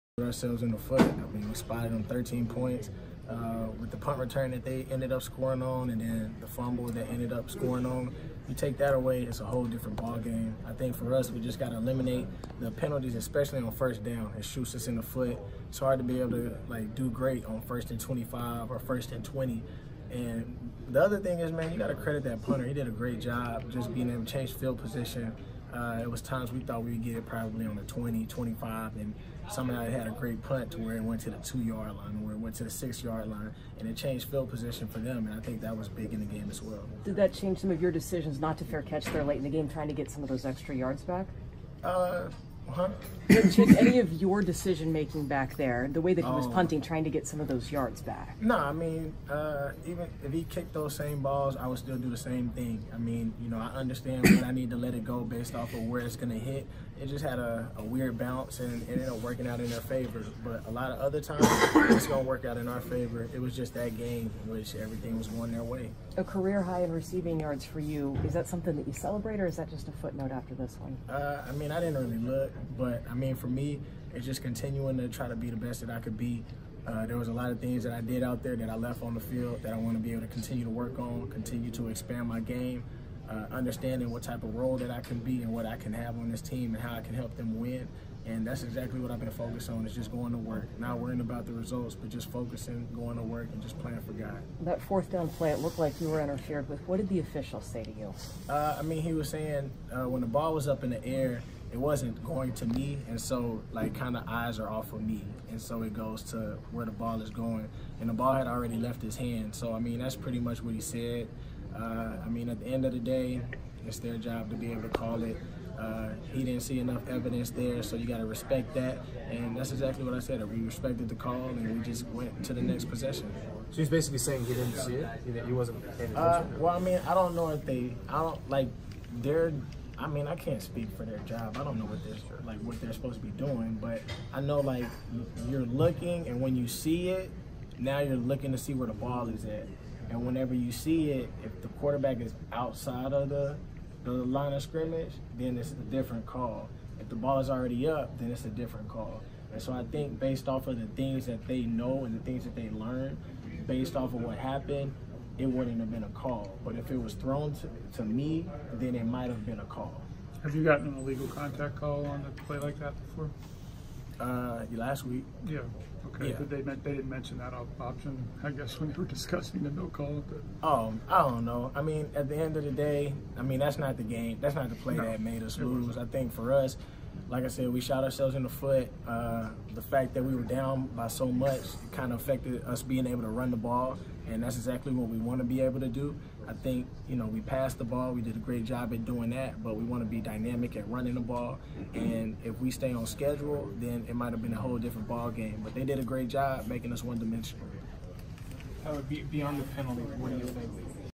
Tyler Lockett says Seahawks better quit giving away games early
Wide receiver Tyler Lockett says after a career day the Seahawks better quit giving away games early and always expect to come back and win. They did. That against Cincinnati and Pittsburgh.  But not against New Orleans.

Video by Gregg Bell/Tacoma News Tribune